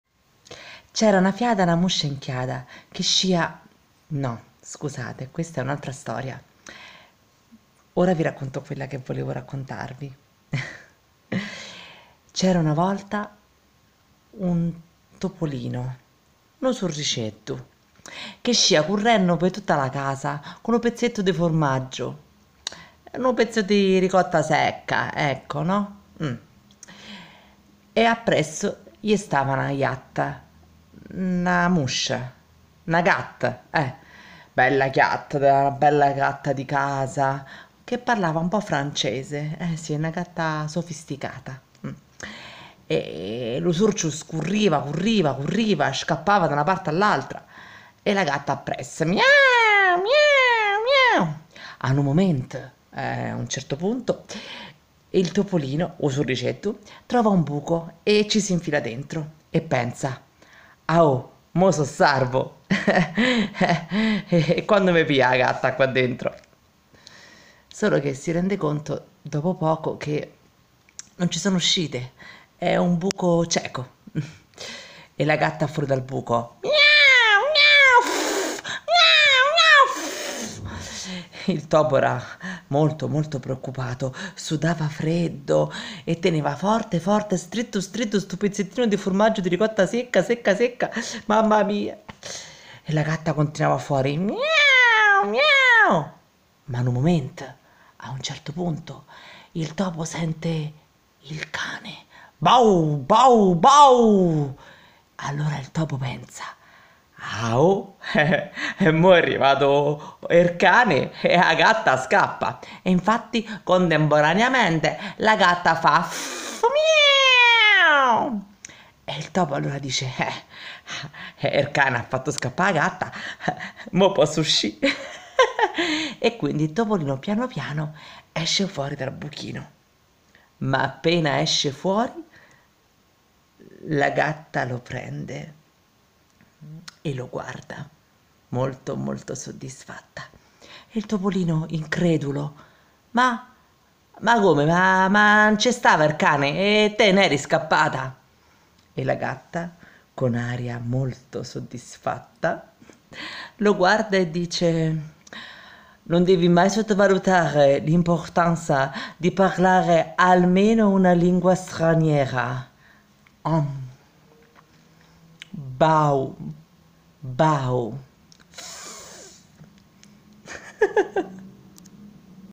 Sara Aprile. Il topo e la gatta.
Video da parte di Sara Aprile. Fiaba ironica con uso di diversi dialetti: salentino .. romano... siciliano... napoletano.. e anche un po' di italiano e accento francese. Ahhhh